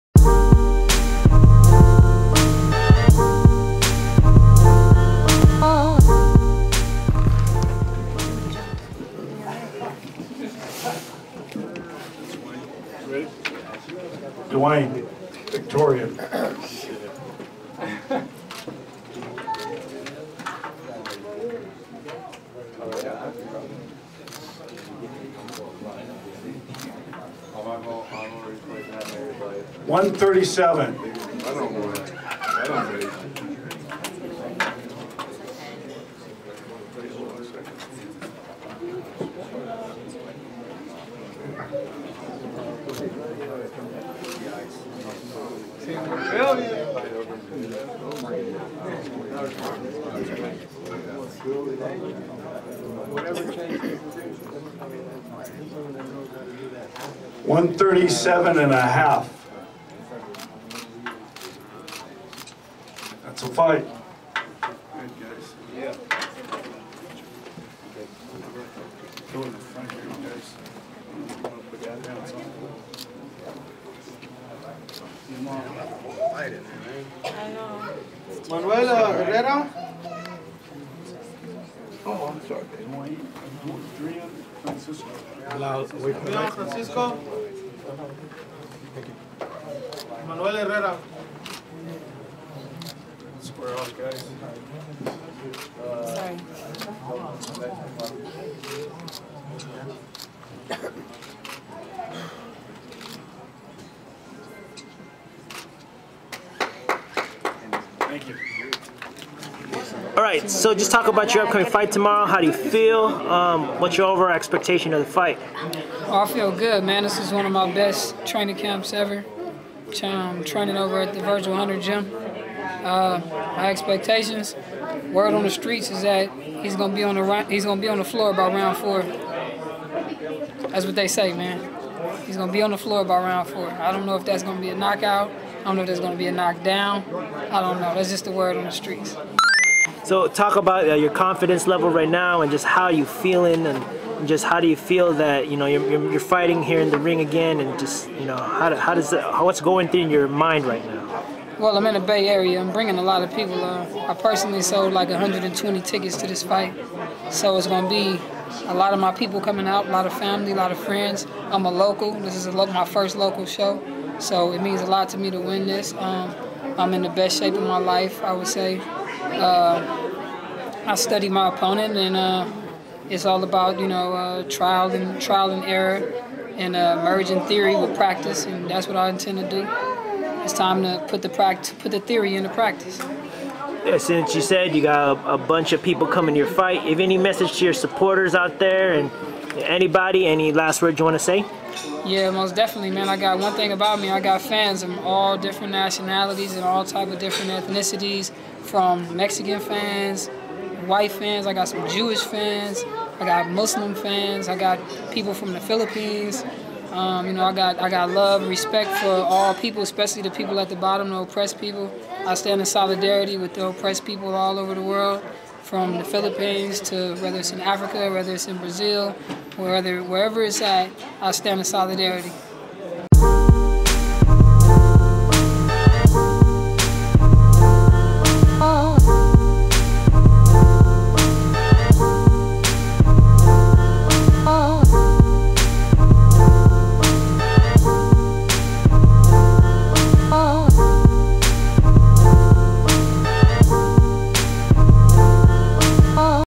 Bilal Mahasin / Dwain Victorian [WeighIn + Interview]
Boxer Bilal Mahasin talks about his upcoming fight with Dwain Victorian.

Bilal Mahasin
Dwain Victorian

Follow me for more inside updates

Shouts out to Lukie for the Instrumental